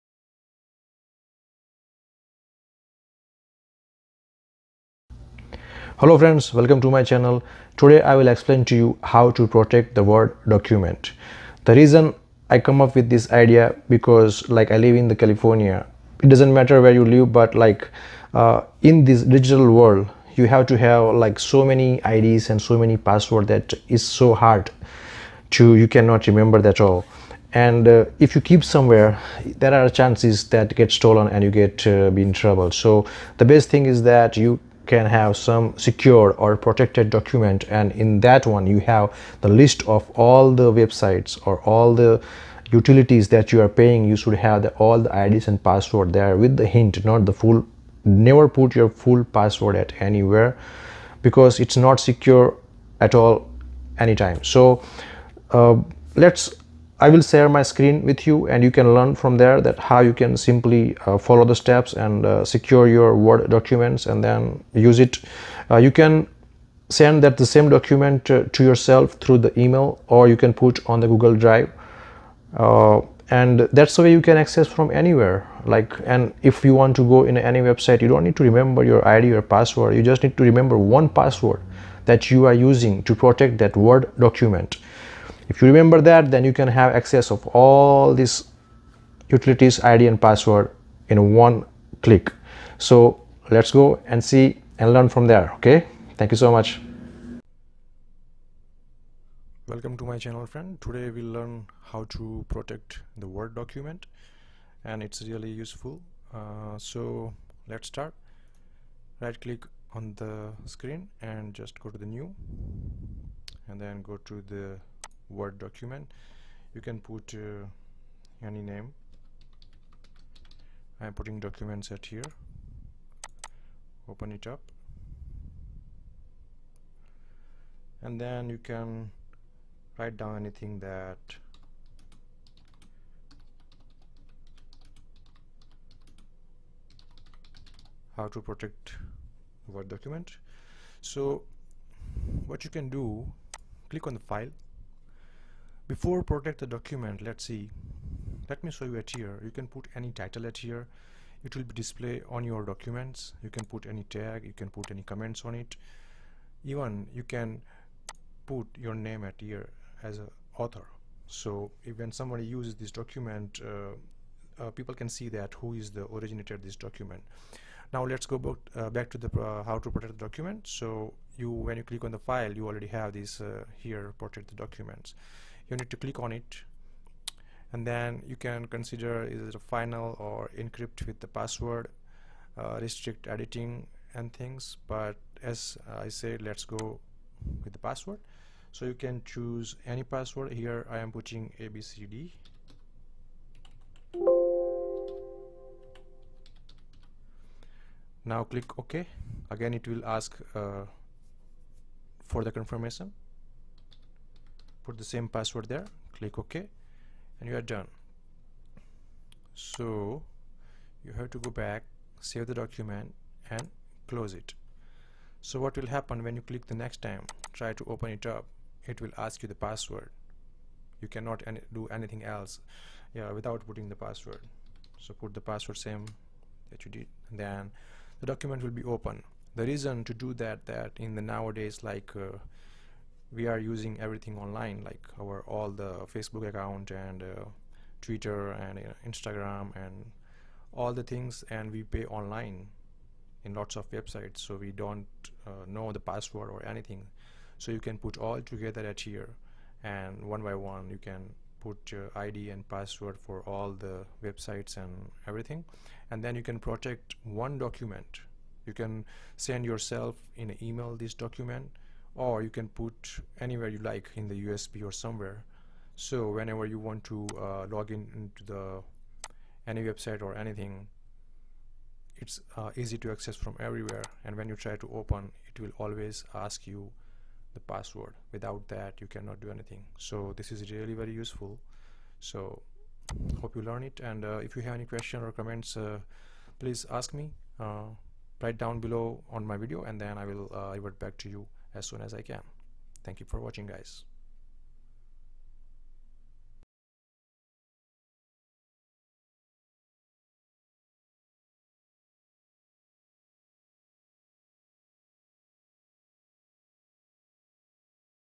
How to Password Protect a Word Documents in Microsoft Word 2010
Want to learn how to protect your word documents with a password, so no one can edit or view them without your permission.

Follow this step-by-step tutorial and Learn How to do so.

Feel free to drop your questions in the comment section.